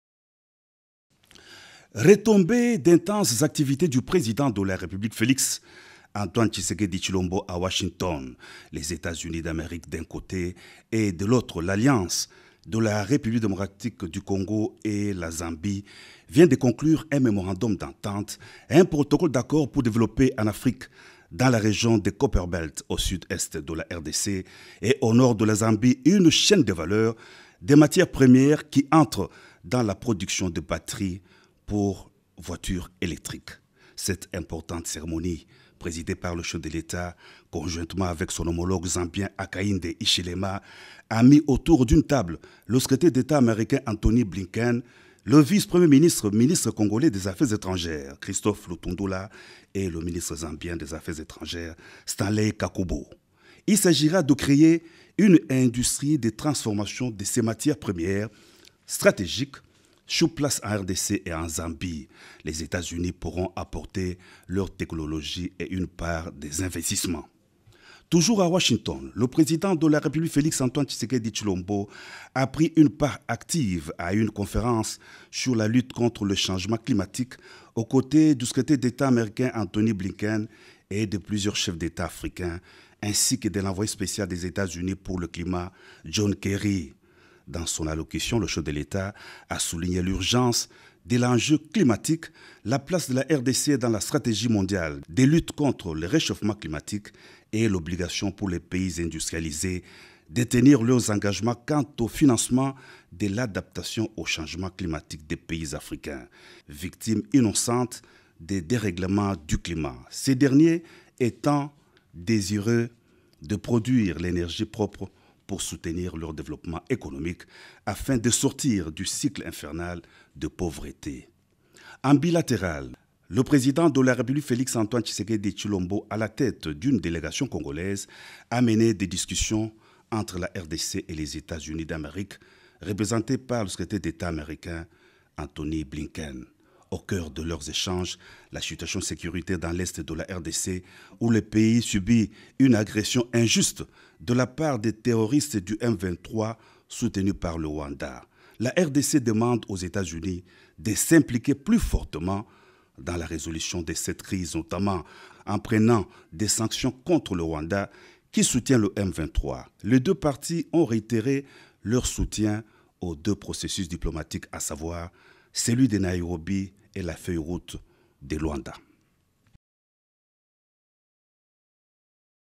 Sommet USA-AFRIQUE: Reportage sur la Signature MOU : USA, RDC, ZAMBIE, Rencontre bilatérale RDC-USA
Sommet USA-AFRIQUE: Reportage sur la Signature MOU : USA, RDC, ZAMBIE, la Conférence sur le climat, l'adaptation, la conservation de la biodiversité et la transition énergétique et la Rencontre bilatérale RDC- USA avec Antony Blinken